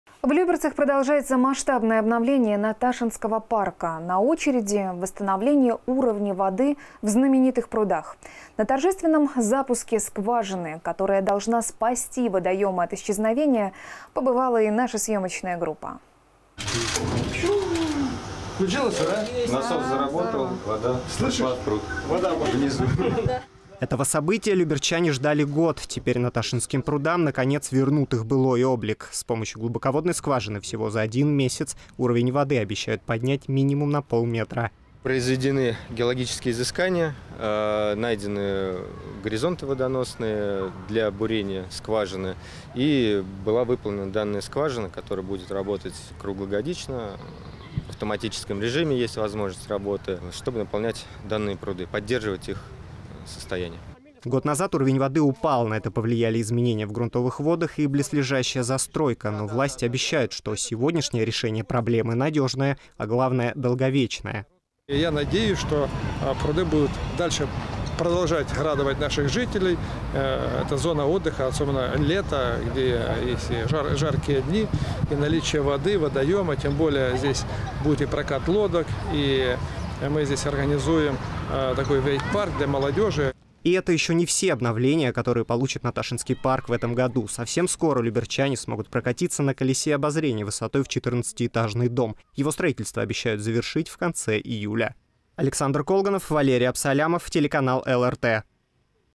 Спасение Наташинских прудов
В Люберцах продолжается масштабное обновление Наташинского парка. На очереди - восстановление уровня воды в знаменитых прудах. На торжественном запуске скважины, которая должна спасти водоемы от исчезновения, побывала и наша съемочная группа.

Этого события люберчане ждали год. Теперь Наташинским прудам, наконец, вернут их былой облик. С помощью глубоководной скважины всего за один месяц уровень воды обещают поднять минимум на полметра.

Вячеслав Волковинский, главный инженер подрядной организации

«Произведены геологические изыскания, найдены горизонты водоносные для бурения скважины и была выполнена данная скважина, которая будет работать круглогодично, в автоматическом режиме есть возможность работы, чтобы наполнять данные пруды, поддерживать их состояние».

Год назад уровень воды упал. На это повлияли изменения в грунтовых водах и близлежащая застройка. Но власти обещают, что сегодняшнее решение проблемы – надежное, а главное - долговечное.

Владимир Ружицкий , глава городского округа Люберцы:

«Я надеюсь, что пруды будут продолжать дальше радовать наших жителей. Это зона отдыха, особенно лето, где если жаркие дни и наличие воды, водоема, тем более здесь будут и прокат лодок и мы здесь организуем такой вейк-парк для молодежи».

И это еще не все обновления, которые получит Наташинский парк в этом году. Совсем скоро люберчане смогут прокатиться на колесе обозрения высотой с четырнадцатиэтажный дом. Его строительство обещают завершить в конце июля.